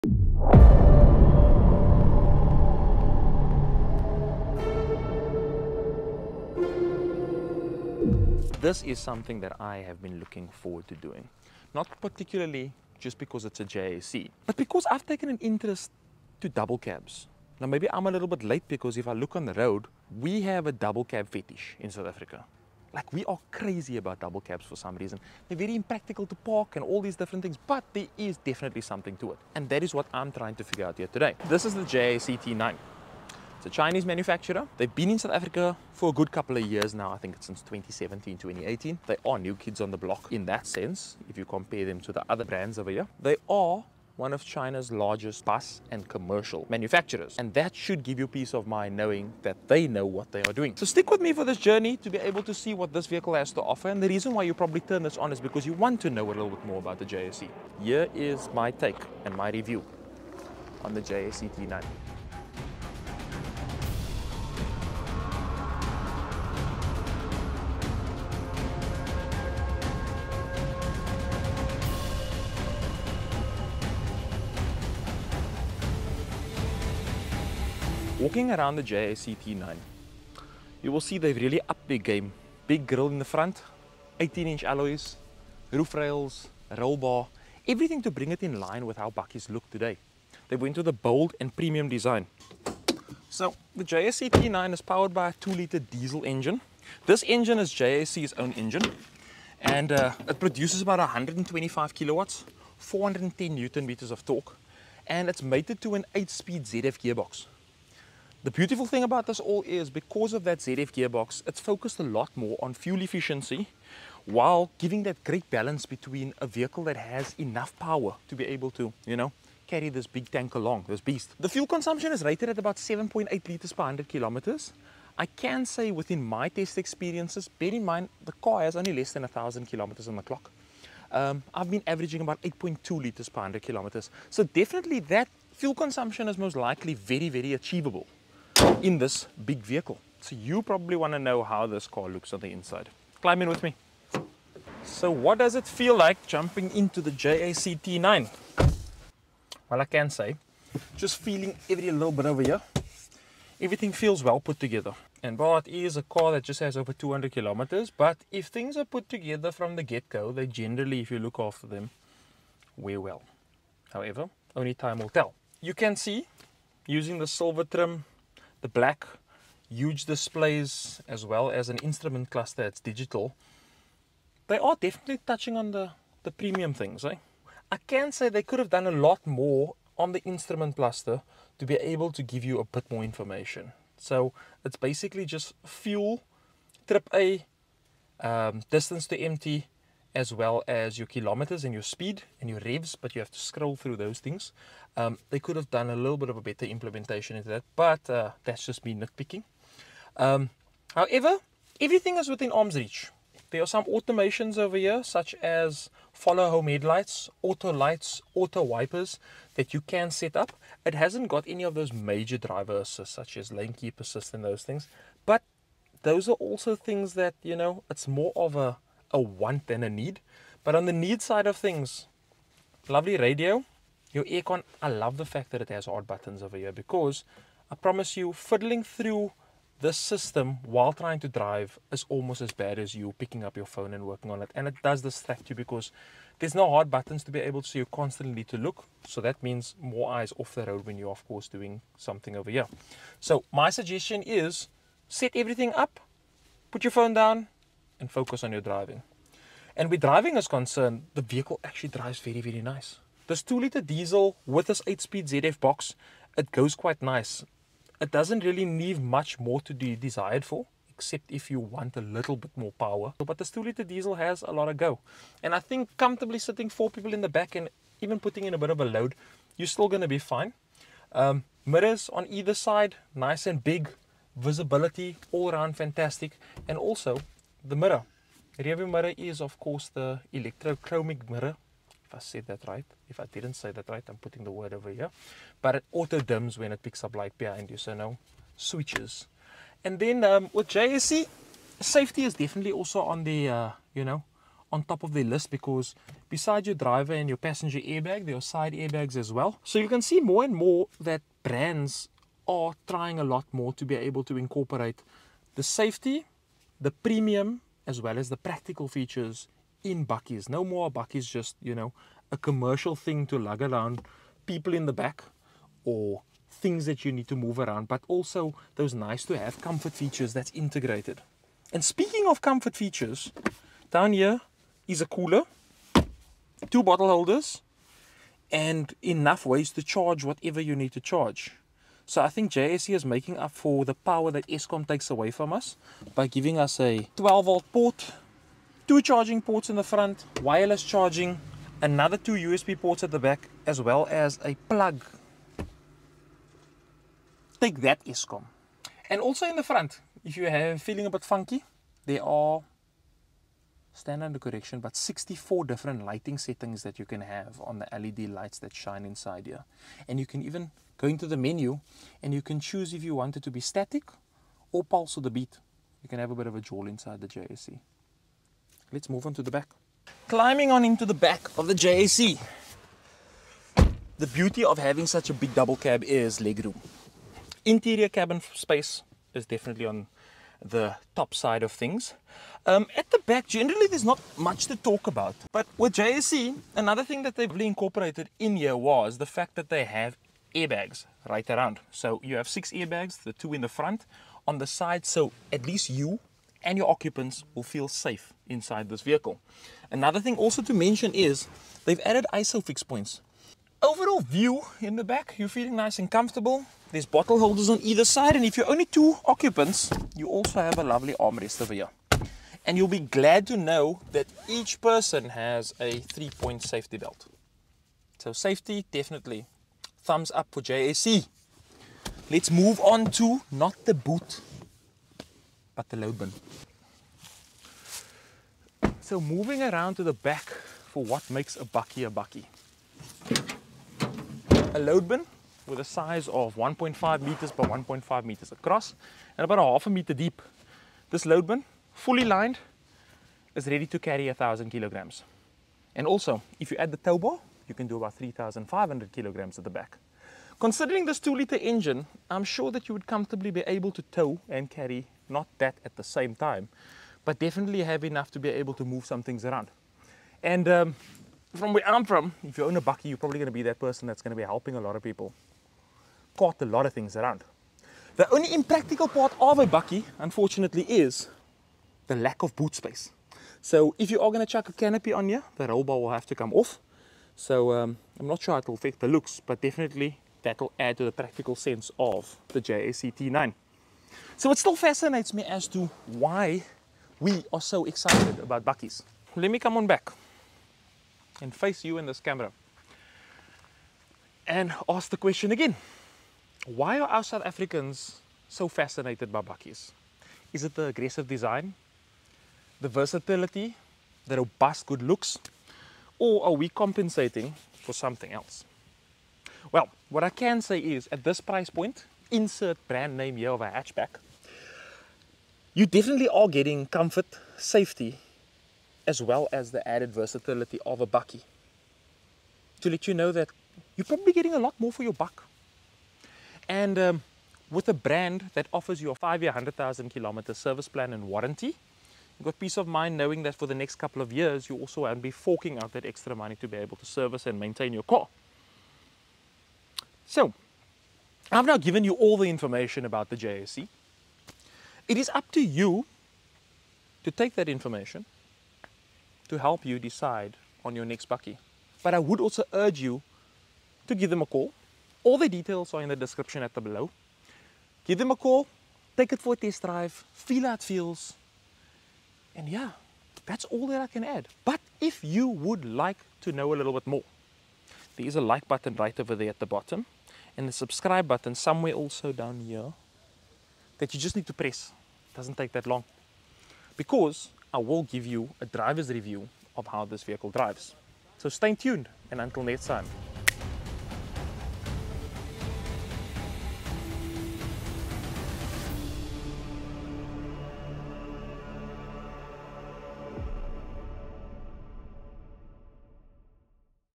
JAC T9 Premium Bakkie at the price of a Hatch Back
The JAC T9 grabbed my attention with its good looks, excellent feature offering and surprising performance. The ride quality was a bit on the hard side, however with leaf spring suspension to help it carry the extra load, its still a great all rounder double cab at a surprisingly decent entry point.

Thanks to

William Simpson JAC Plumstead - Address:  15A Main Rd, Plumstead, Cape Town, 7800

Assistance - Rod Dingle & Alexis Johannes (Camera and Audio)